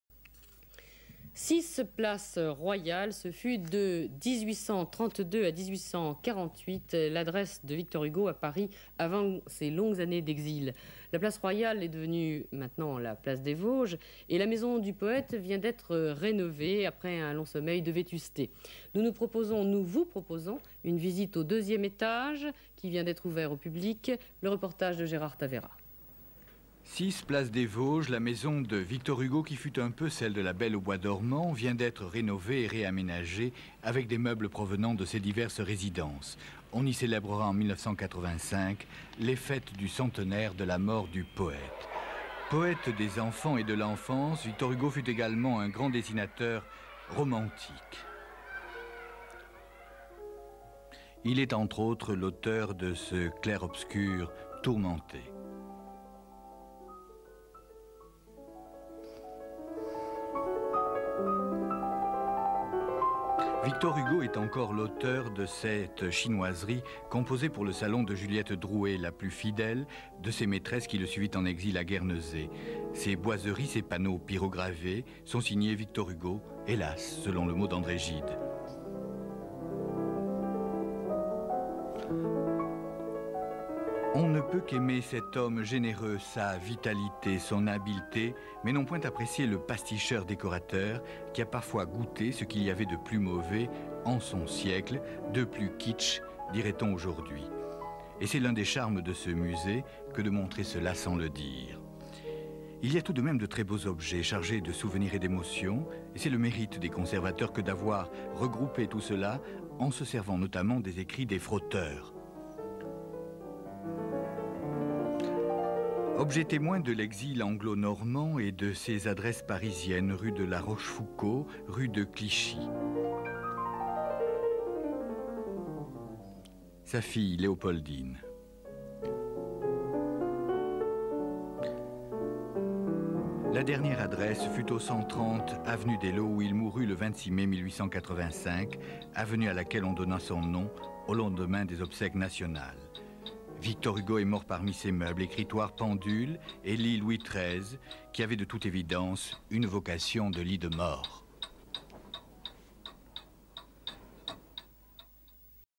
Victor Hugo’s house in Paris | INA Archive
10th December 1983
Place des Vosges in Paris, the house of Victor HUGO has just been restored and transformed into a museum. Comments on the view over the square and on the visit of the museum. Presentation of objects and masterpieces (images of “Victor Hugo in his living room at 21 rue de Clichy in 1875”)

10 décembre 1983
Place des Vosges à Paris, la Maison de Victor HUGO vient d'être restaurée et réaménagée en musée. Commentaires sur des vues de la place et sur la visite du musée. Présentation des objets et oeuvres d'art (images de"Victor Hugo dans son salon du 21 rue de Clichy en 1875").